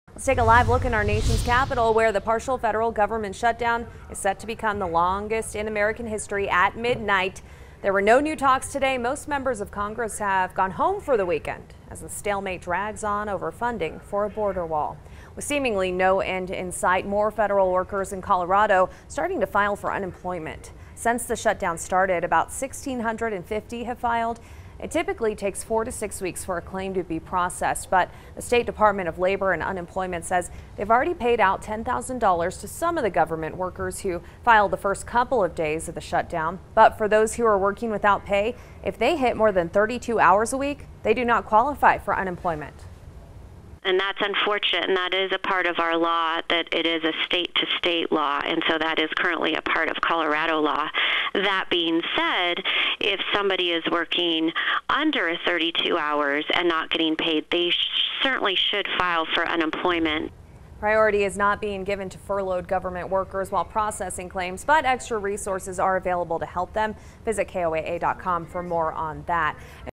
1,600+ federal government employees file for unemployment in Colorado
Over 1,600 federal workers in Colorado have filed for unemployment, since the start of the partial government shutdown.